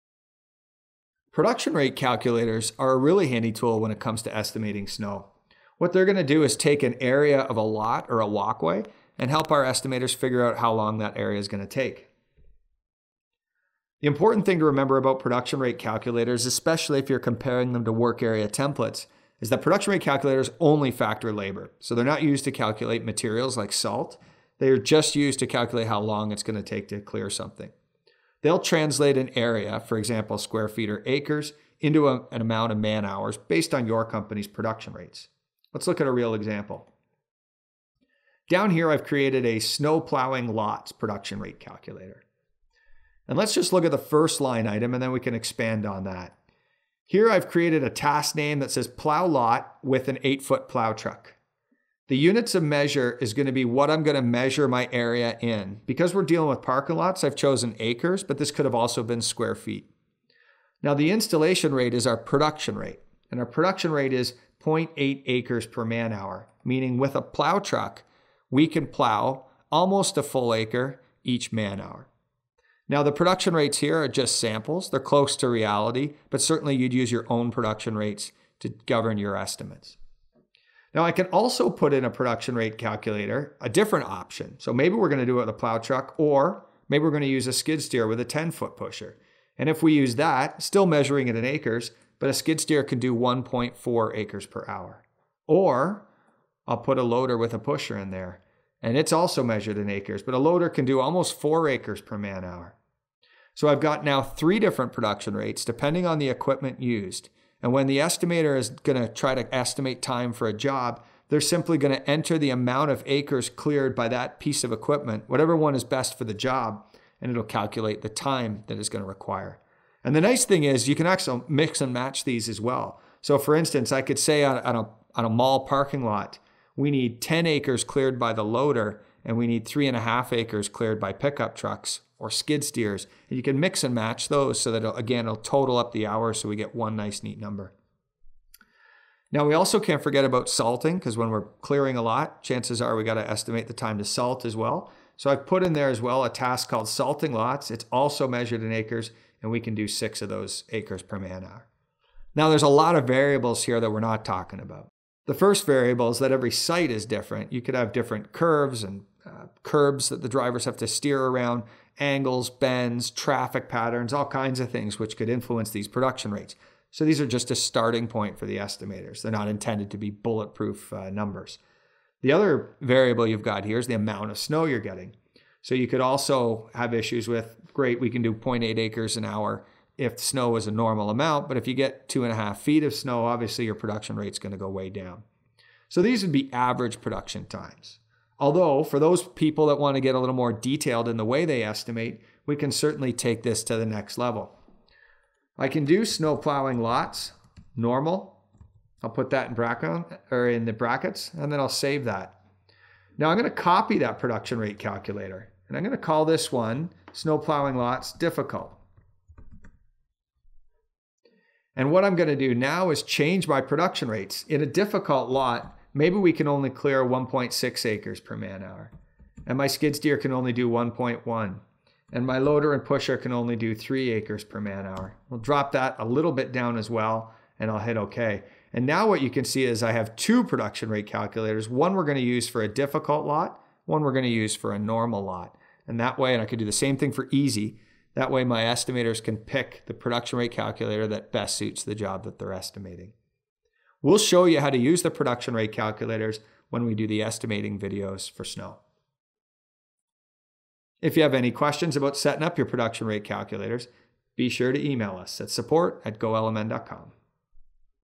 LMN - Production Rates for Snowplowing
In this video, you'll learn how to production rate calculators for snow and ice using LMN's estimating software.  The calculators help to ensure fast, consistent, profitable estimates across anyone in your company who is bidding work.  In just a few minutes, you can setup a calculator that will establish snowplowing times (e.g. acres per hour) for a variety of different equipment and/or site complexity scenarios.

If you're looking for a more efficient way to price and manage snow operations, then check out LMN's estimating and mobile timesheet software.